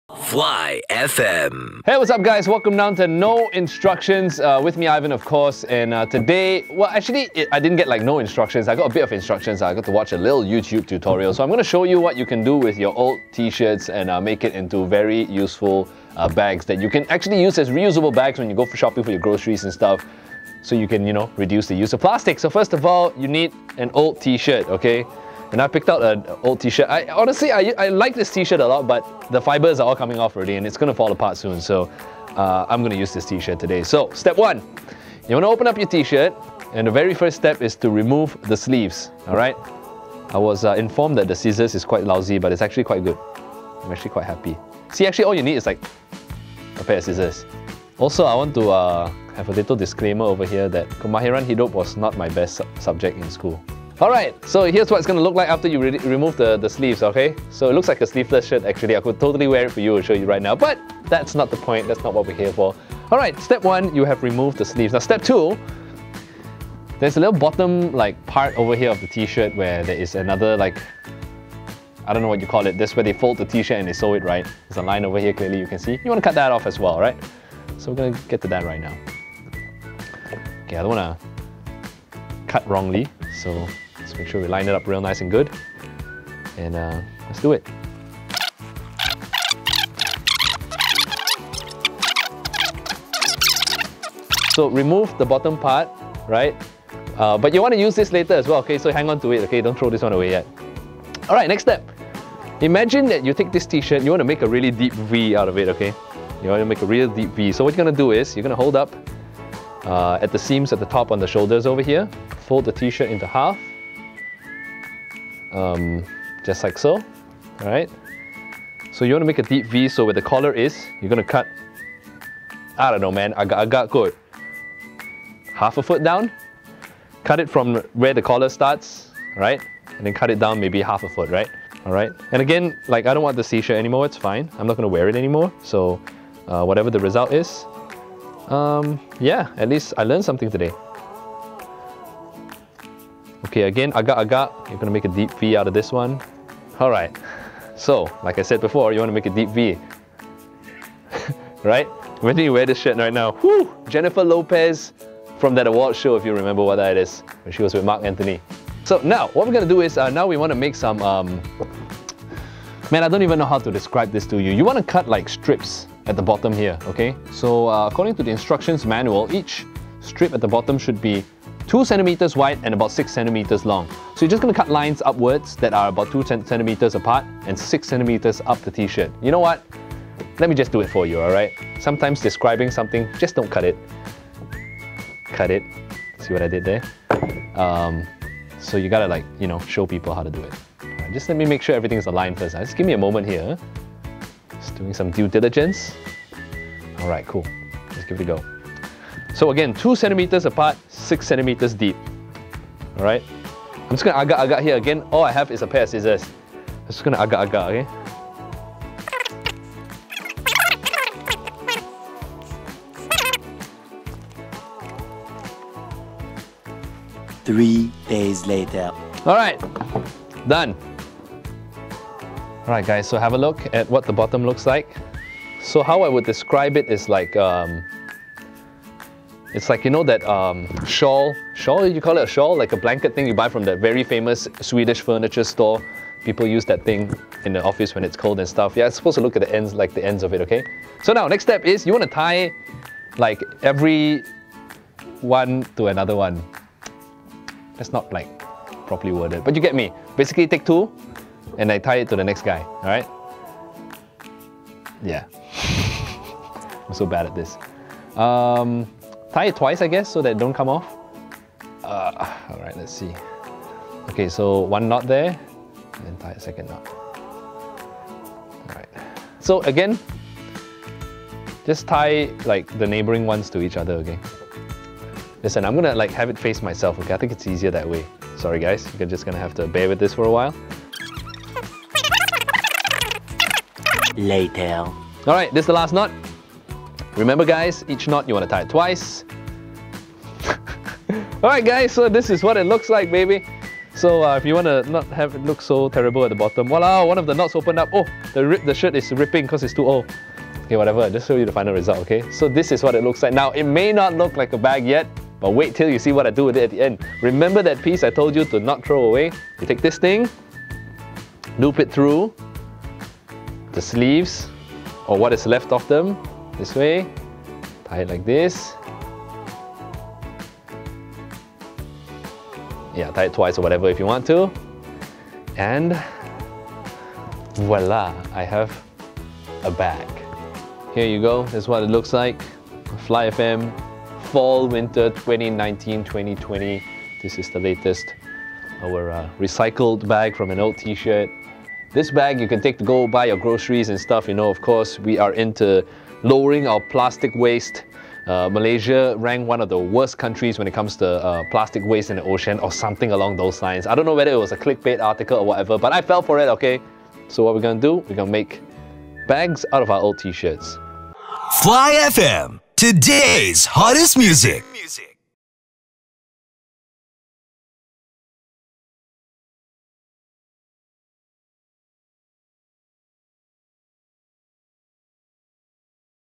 No-Sew T-Shirt to Tote Bag DIY!
Ivan tries to DIY a Tote Bag with ZeroInstructions !
Check out how you can re-use your own t-shirts and make them into reusable tote bags!